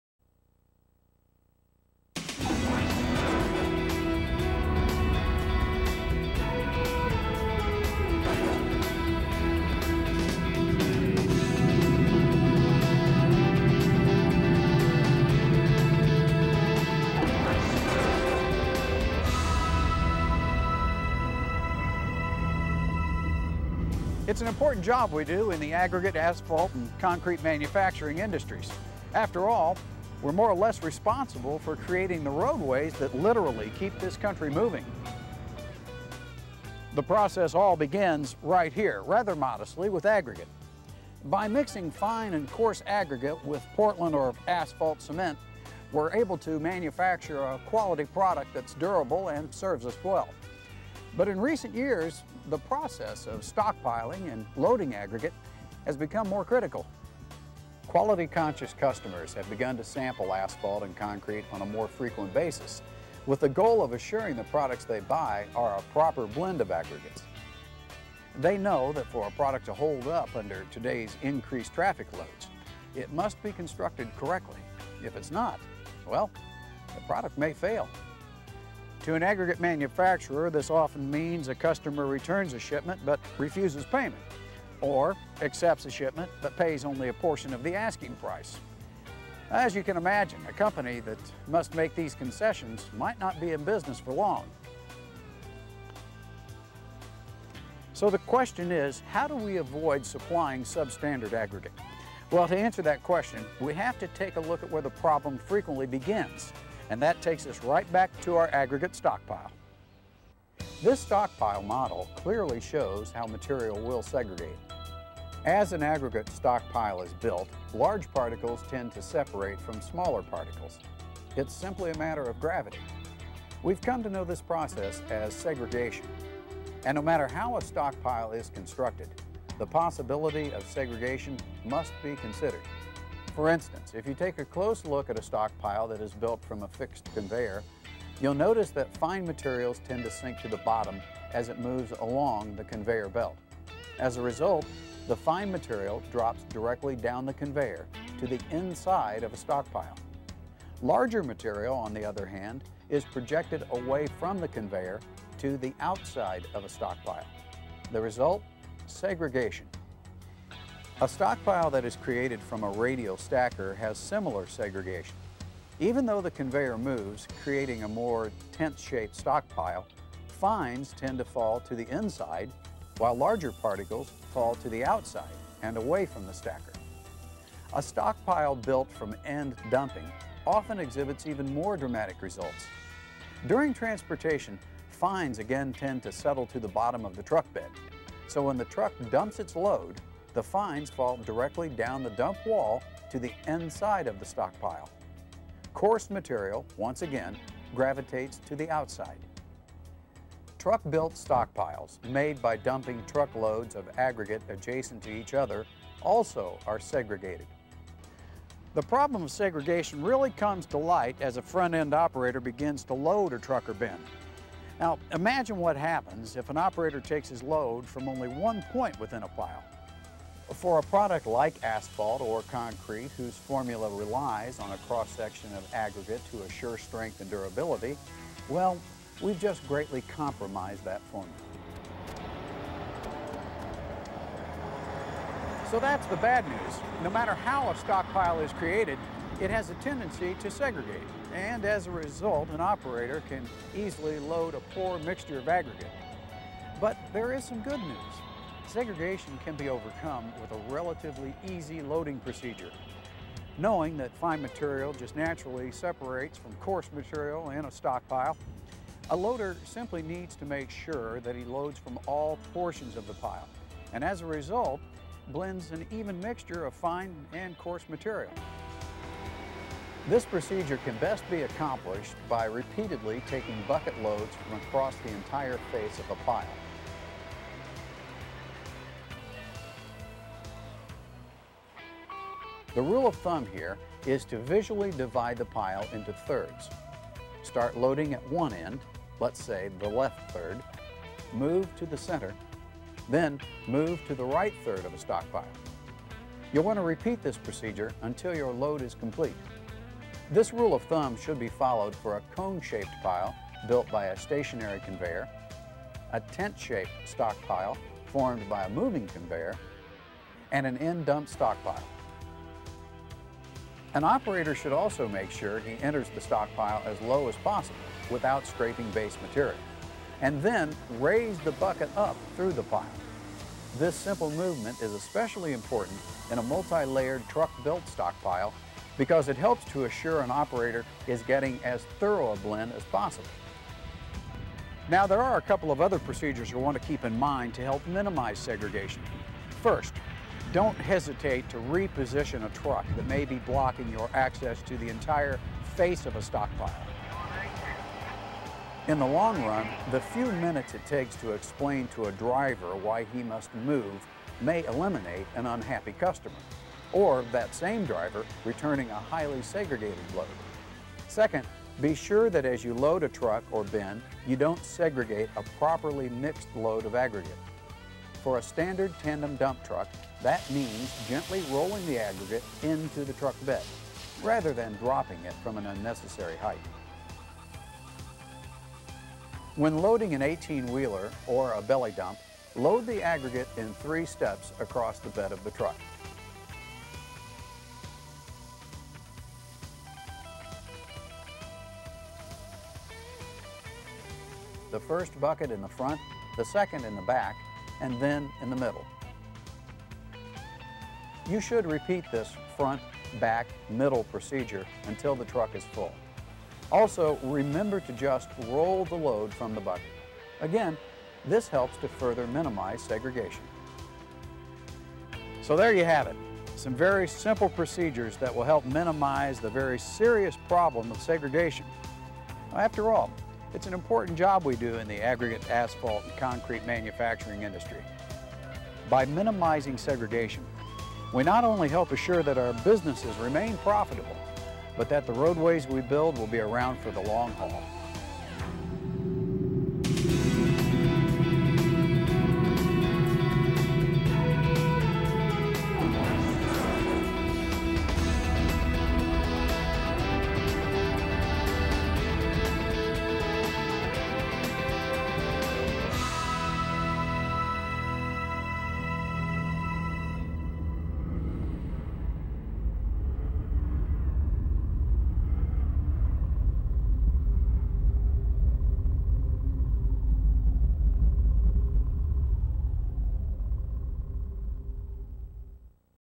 Aggregate Stockpile Recovery
An informational video about aggregate segregation in stockpiles and how to minimize the effects when loading for transport.
U.S. Department of Transportation-1999